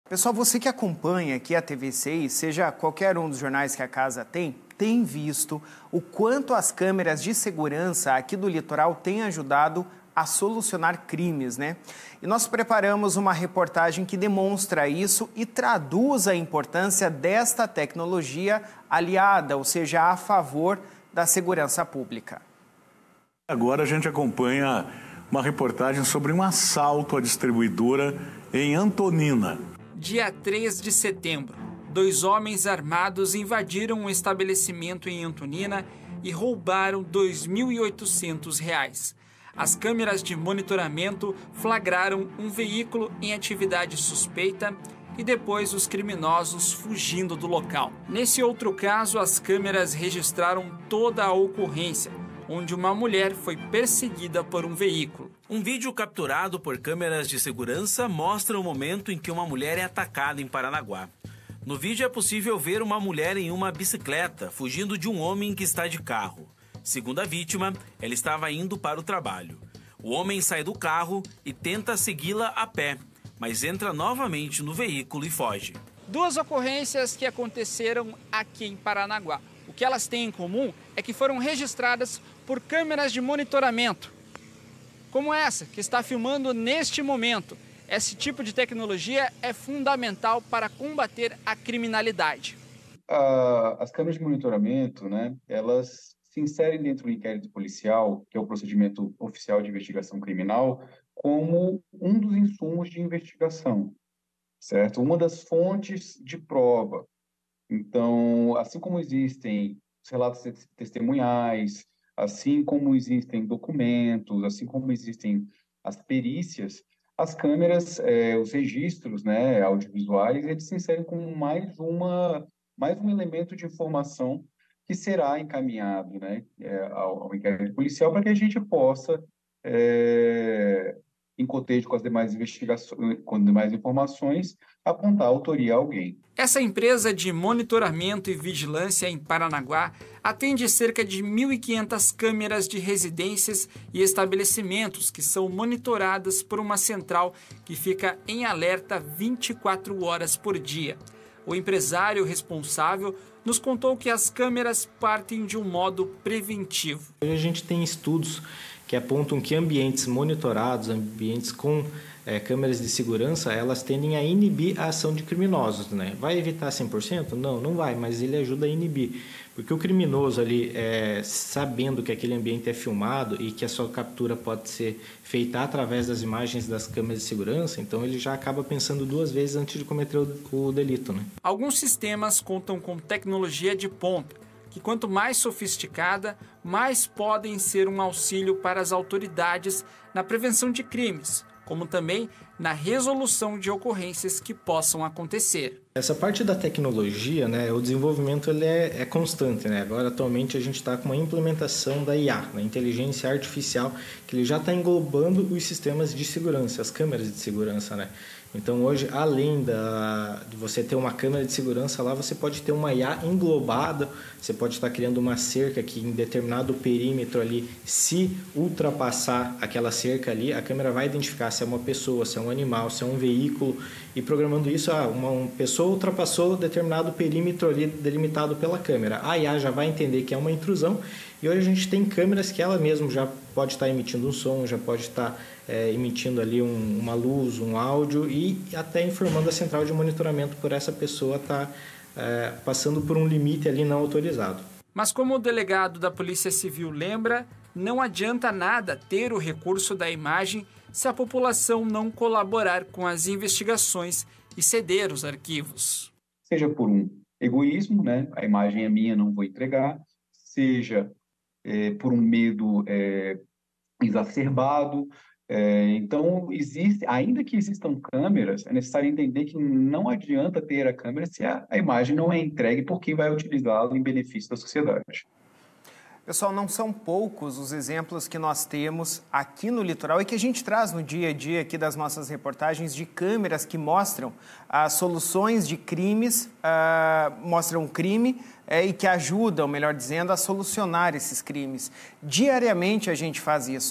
TECNOLOGIA A FAVOR DA SEGURANÇA
Câmeras desempenham papel fundamental na resolução de crimes